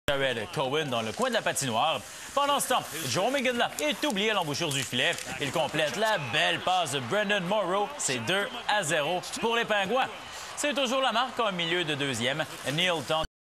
Brenden Morrow nice passe on Jarome Iginla Vs Ottawa ( April 22nd)
Nice Litle passe from Morrow on Iginla's goal !